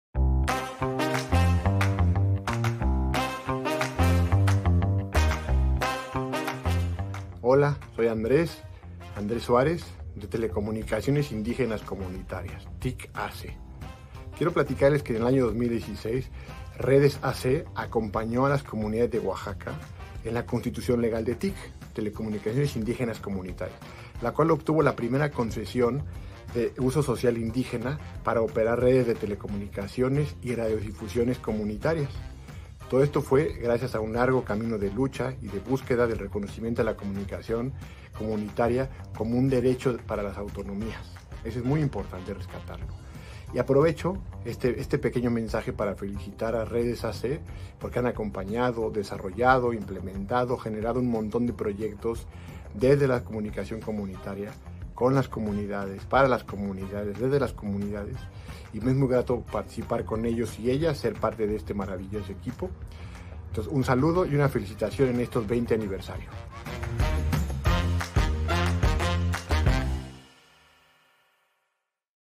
2016: Concesiones de uso social indígena 20añosaCOMPAñando 🐚
📲📡  ¡Continuamos contando la historia! Después de utilizar una concesión experimental por dos años, 16 comunidades indígenas de Oaxaca decidieron, junto con REDES A.C. y Rhizomatica, constituirse bajo la figura legal de Telecomunicaciones Indígenas Comunitarias A.C. (TIC A.C.) obteniendo así la primera concesión social indígena en la historia de México para administrar y operar redes de telecomunicaciones y radiodifusión autónomas, entre ellas, la telefonía celular. Esto fue el resultado de un largo camino de lucha por el derecho a la comunicación y la autonomía de los pueblos indígenas.

Conoce más del procedimiento para solicitar una concesión en nuestra página web